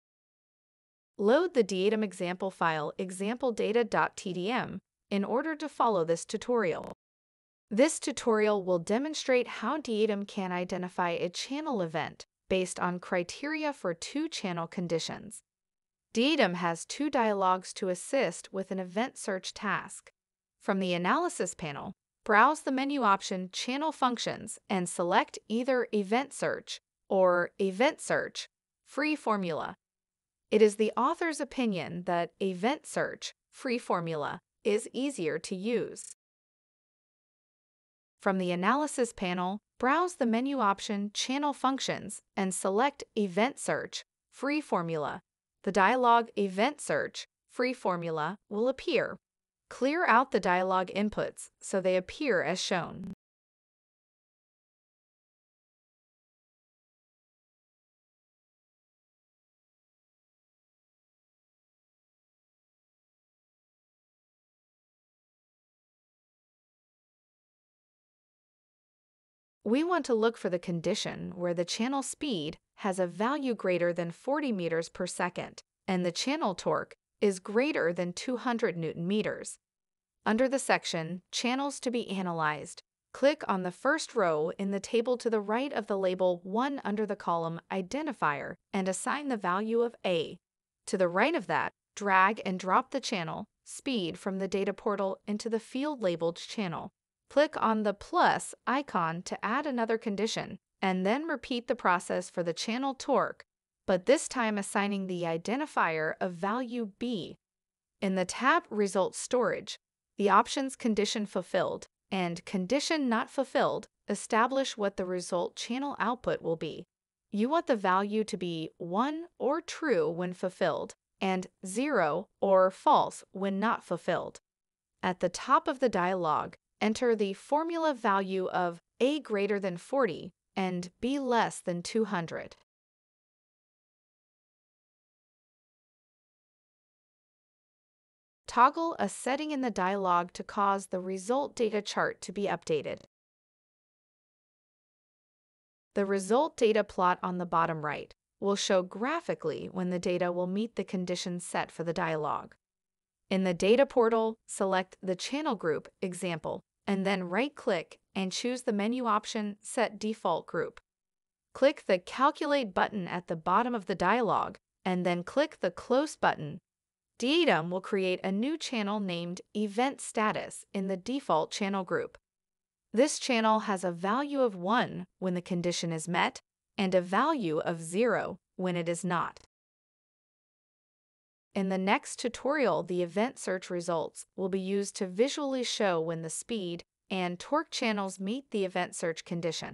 NI DIAdem tutorial #28 - ANALYSIS Finding A Channel Event
Learn how to locate channel events based on criteria for two or more channel value conditions.